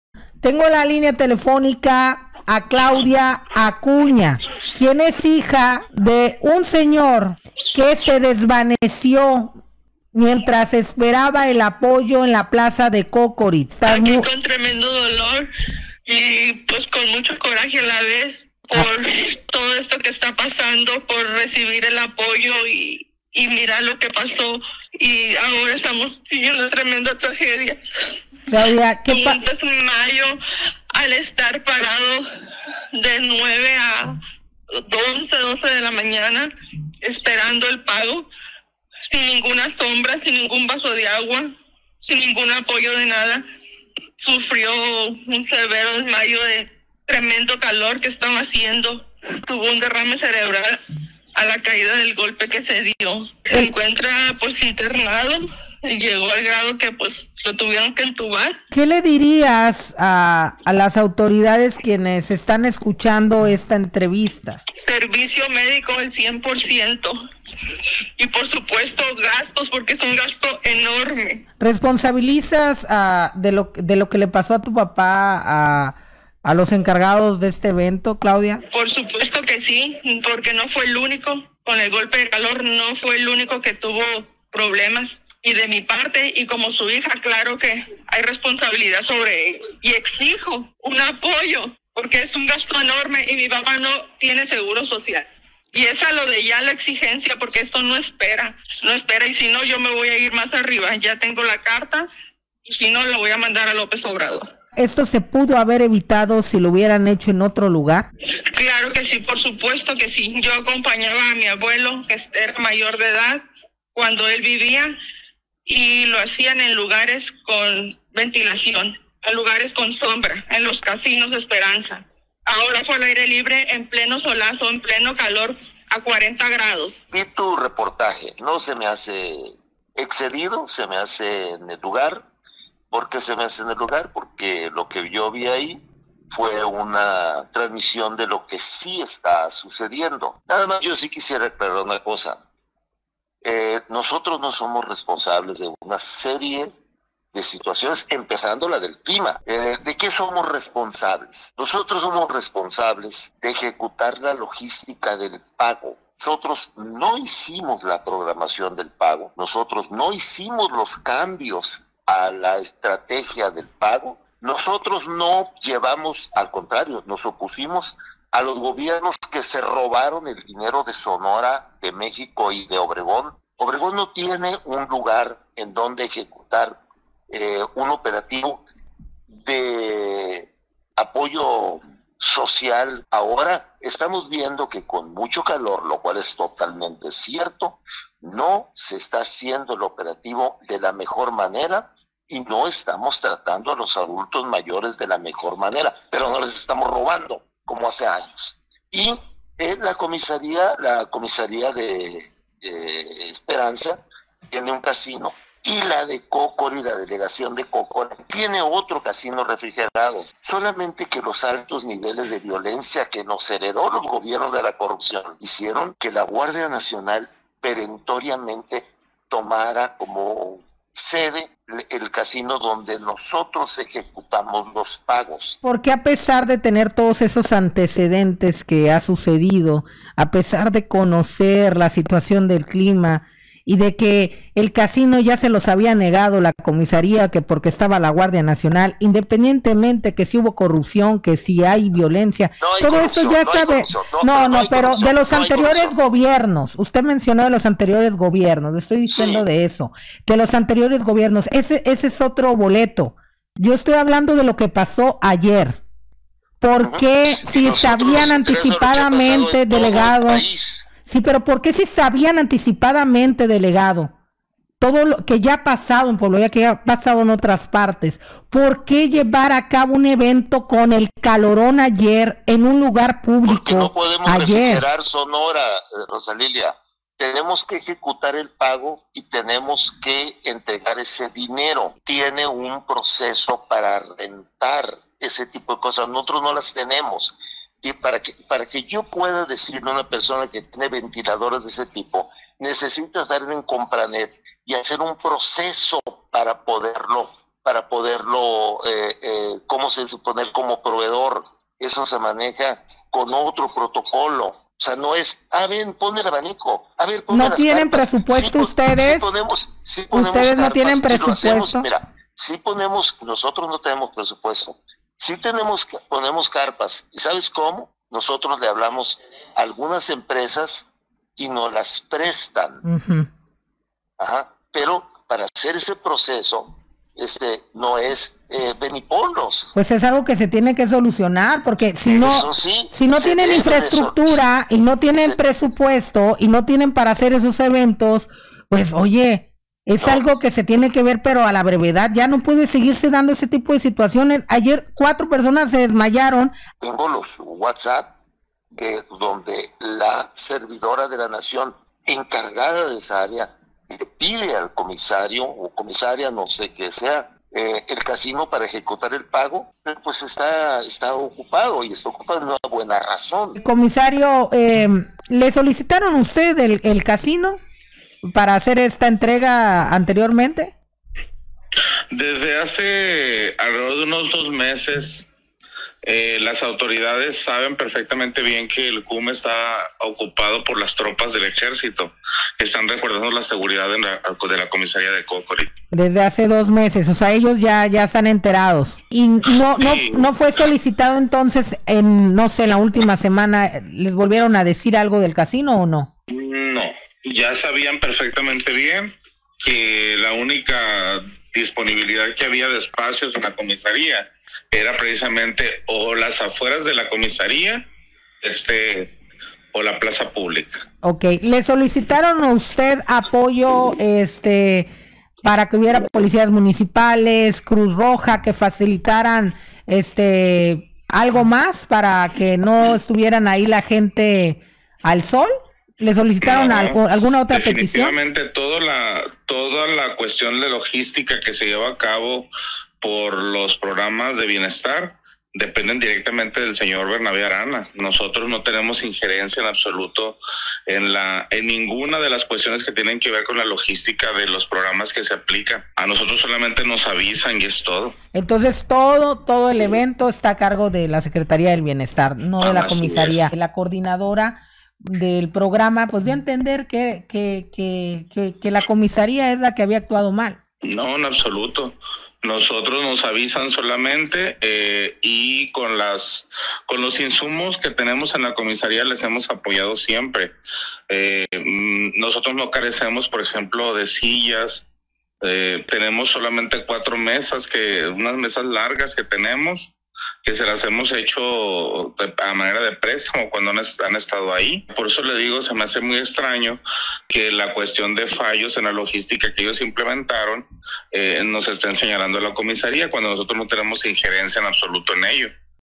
SUFRE RAFAEL DERRAME CEREBRAL POR CULPA DEL CALOR EN ENTREGA DE APOYOS BIENESTAR SONORA
La necesidad de cobrar un dinero que le hace falta y que tiene por derecho al ser adulto mayor, significó para Rafael Acuña Acosta de 67 años el camino a un destino inesperado e incierto que hoy enfrenta su familia.